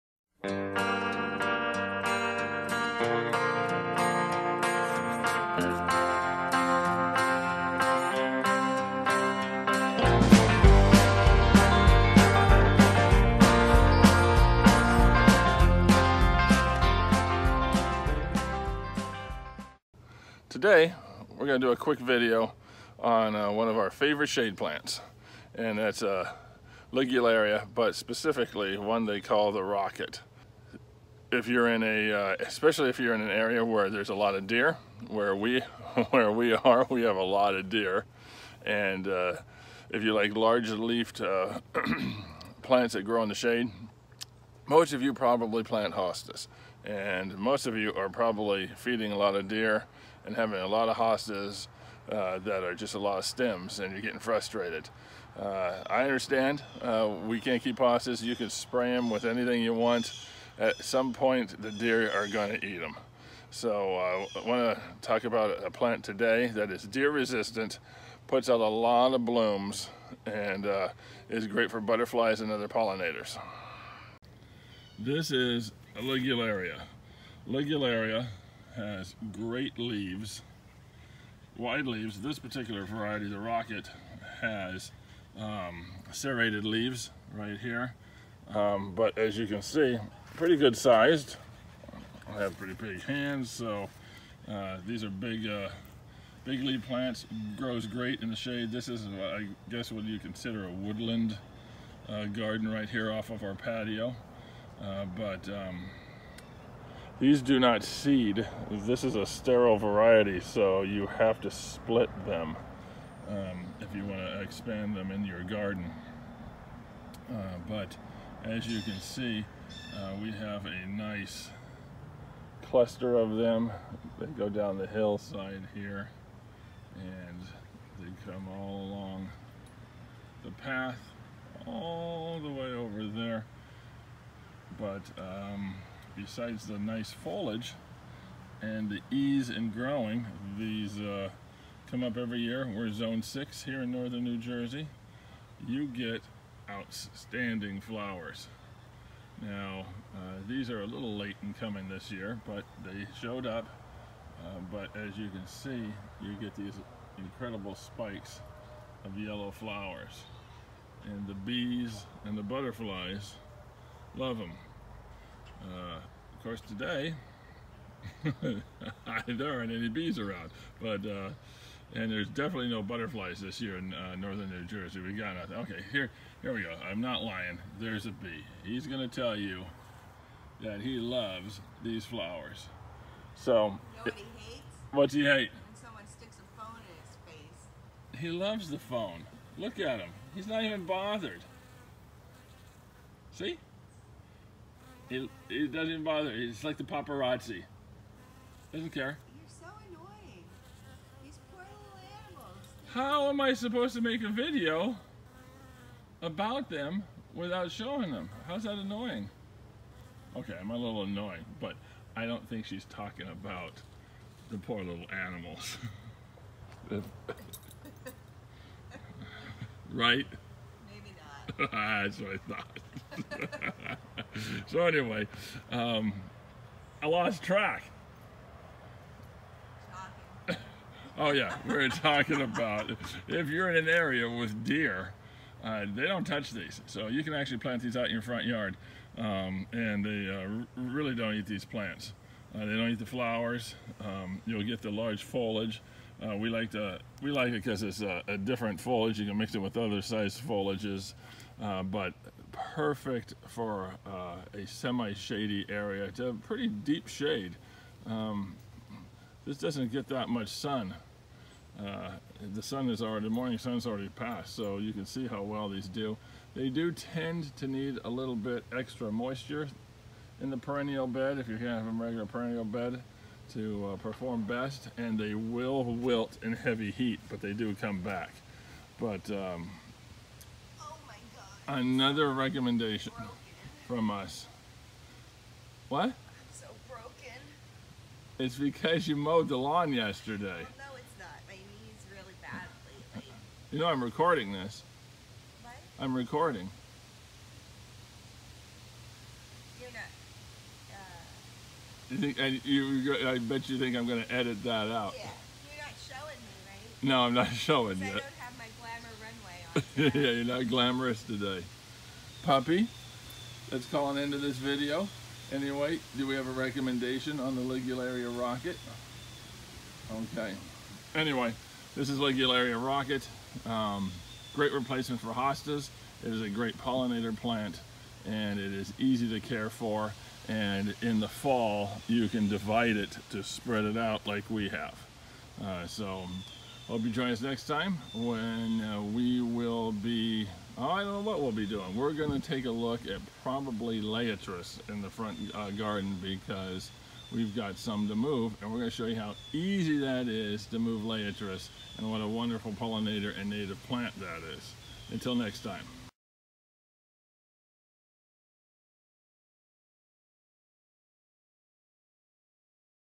Ligularia "The Rocket"
One of our favorite shade plants. Big Leaves, great flowers and best of all, deer resistant. We love Ligularia " The Rocket".